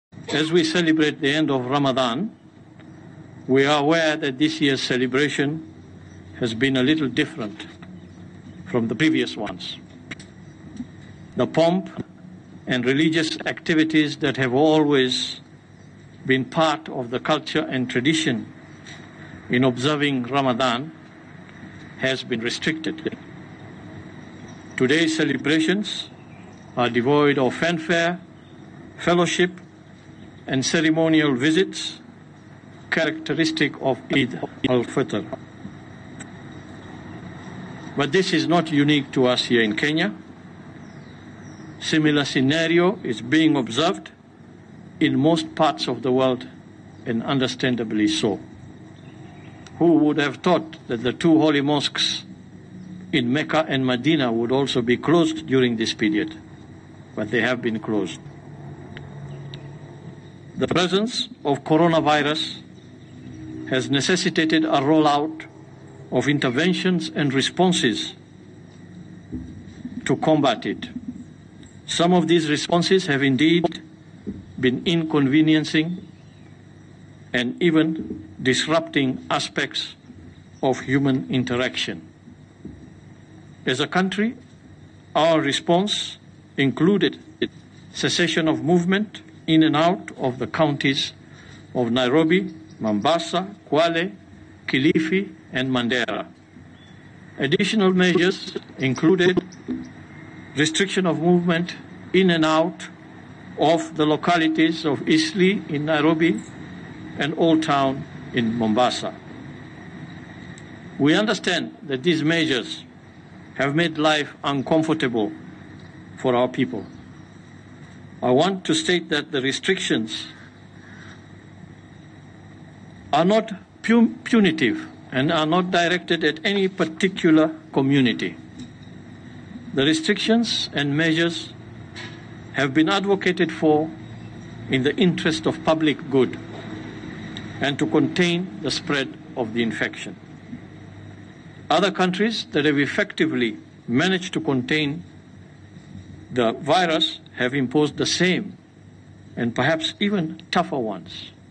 Dr. Aman: Today’s celebrations are devoid of fanfare, fellowship and ceremonial visits
Dr. Aman: Today’s celebrations are devoid of fanfare, fellowship and ceremonial visits characteristic of Eid